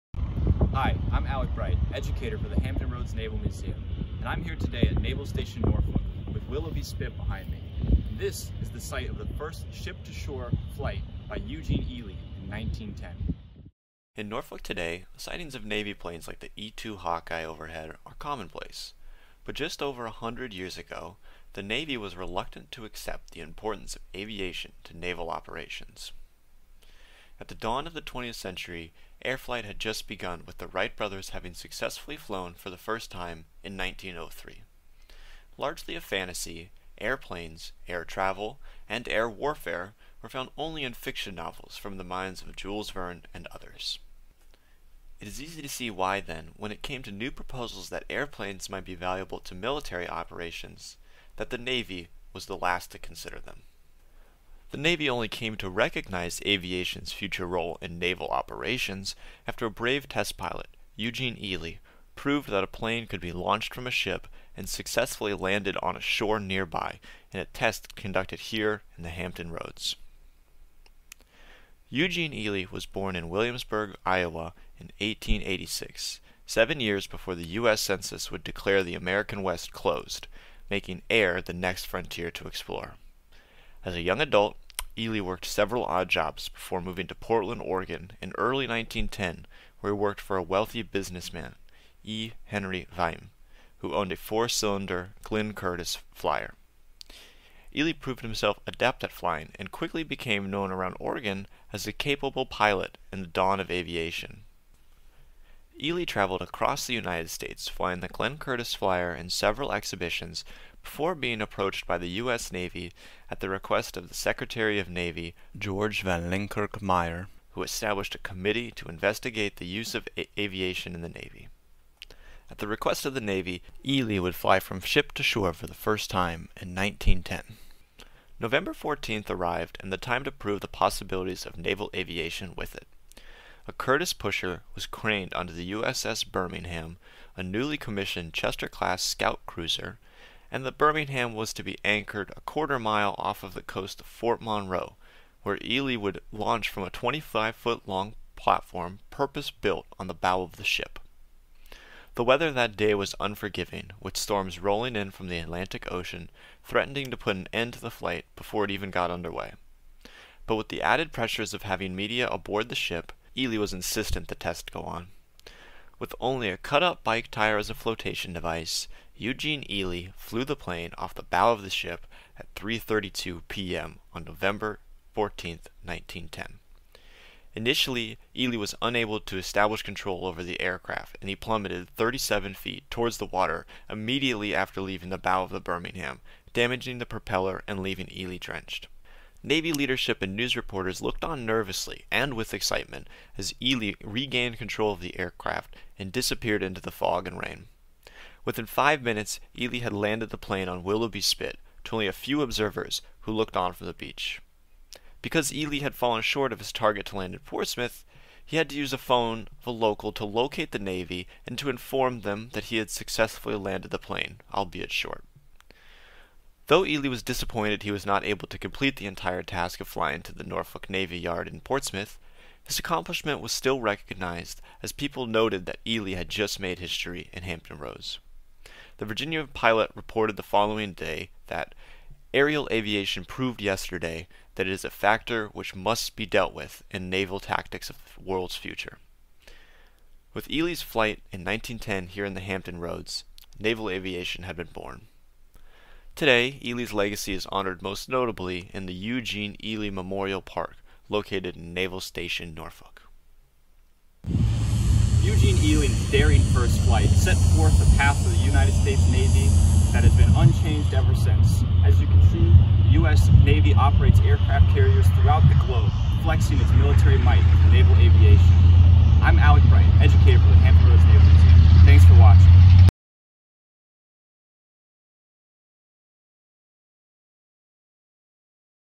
Local History Spotlight: Eugene Ely
In this local history spotlight, HRNM Educator Alec Bright visits the location of the first ship-to-shore flight, conducted by Eugene Ely.

This video is produced, directed, and written by staff at the Hampton Roads Naval Museum.